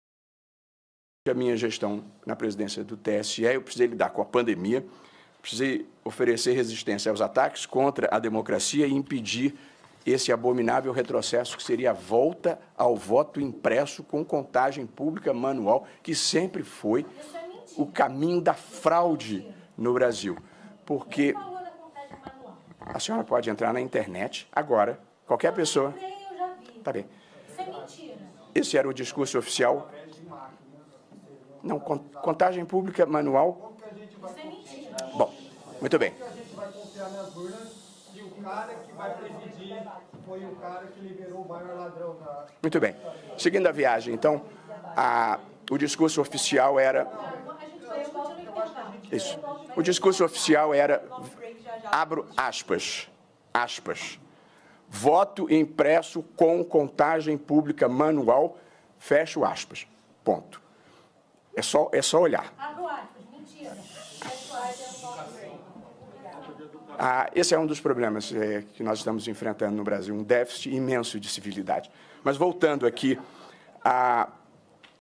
Barroso sendo mentiroso nos EUA.
Ministro ouviu o que merecia.